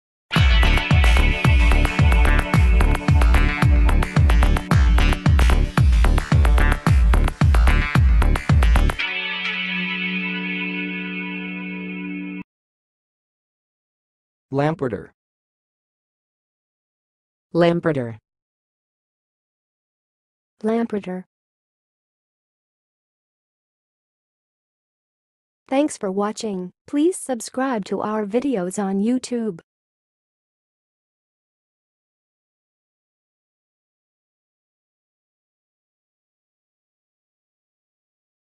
LAMPARTER - HOW TO SAY LAMPARTER?
LAMPARTER - Don't mispronounce Lamparter anymore in 2023

How do you correctly pronounce the surname Lamparter?
Can you provide a phonetic breakdown of Lamparter's pronunciation?
Are there any alternative ways to pronounce the surname Lamparter?
LAMPARTER Meaning And Definition:
Lamparter is a surname of German origin.
It is derived from the Middle High German word 'lampart', which means 'leopard'.
Lamparter is a relatively rare surname and is often found among individuals of German or Swiss descent.
LAMPARTER Pronunciation:
To pronounce 'Lamparter', start with the 'L' sound as in 'leopard', followed by the short 'a' as in 'cat'.
The 'm' sound is next, followed by the 'p' sound as in 'pat'.
The 'ar' sound is pronounced as in 'car', and finish with the soft 't' sound as in 'pet'.
Use LAMPARTER In Your Speech:
1. The Lamparter family has a long history in Germany
2. Do you know anyone with the last name Lamparter?
3. Lamparter is derived from the German word for leopard
Please answer in comments for the most popular LAMPARTER questions:
1. What is the meaning of the surname Lamparter?
2. How is Lamparter pronounced in German?
3. Are there any famous people with the last name Lamparter?